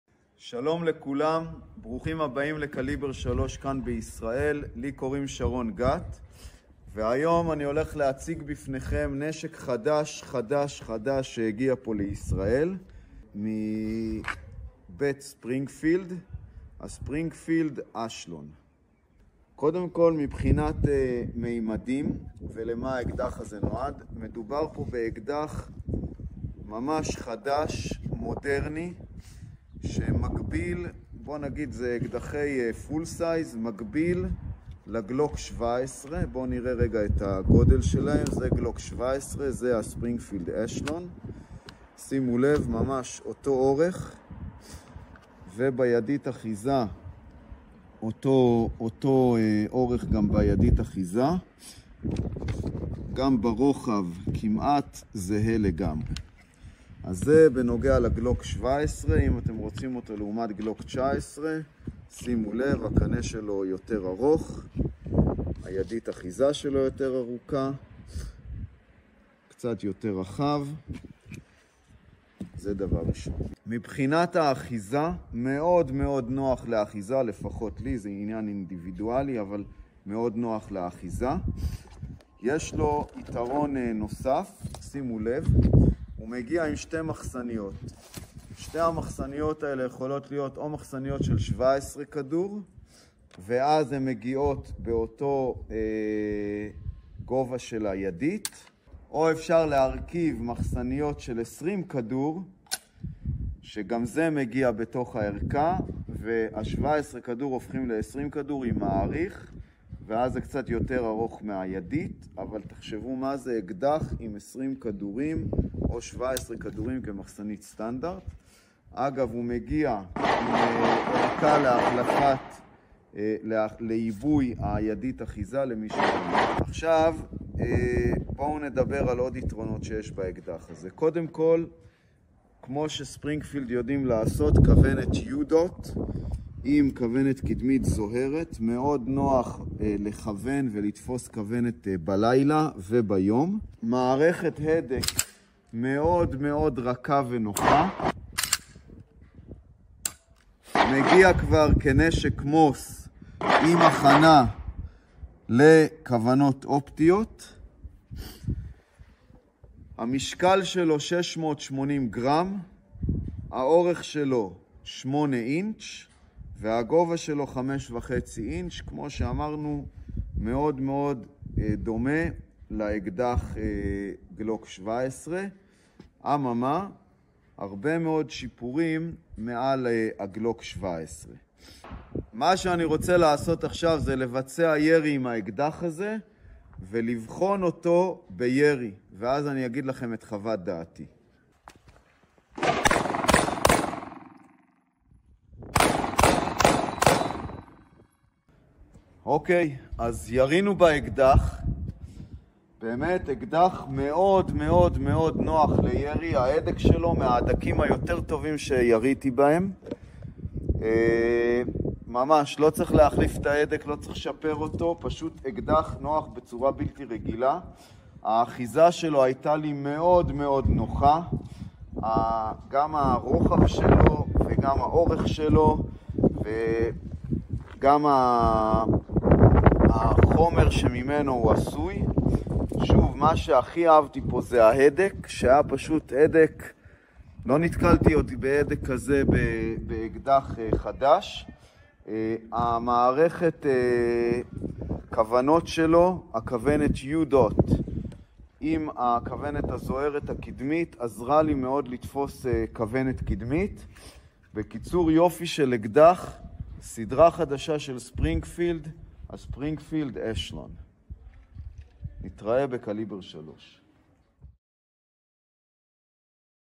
Springfield Echelon Full Review by Col. Sharon Gat
Springfield Echelon Full Review by Col. Sharon Gat Join Caliber 3's own Col. Sharon Gat in this review of the Springfield Echelon, a pistol that impresses straight out of the box. This American-made firearm holds high standard in quality and performance, offering enthusiasts of the Glock platform an exciting alternative. If you're seeking immediate upgrades and enhancements without sacrificing reliability, the Echelon is tailor-made for you.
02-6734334
שימו לב לסקירה של האקדח החדש בישראל- Springfield Echelon.
 אל"מ שרון גת מנכ"ל  קליבר 3 מתנסה ומחווה את דעתו על ה Springfield Echelon:
* אקדח מרשים שדומה בממדיו לגלוק 17.
* . כלי נשק זה מתוצרת אמריקאית מחזיק בסטנדרט גבוה באיכות ובביצועים, ומציע לחובבי פלטפורמת גלוק אלטרנטיבה מרגשת.
* הדק קל ונח לתפעול מהפעם הראשונה, אחיזה נוחה, מחסניות עם 17 כד'.
* אם אתה מחפש שדרוגים ושיפורים מיידיים מבלי לוותר על האמינות, ה-Echelon מותאם במיוחד עבורך.
מוזמנים  לקליבר 3, להתרשם מה Springfield Echelon.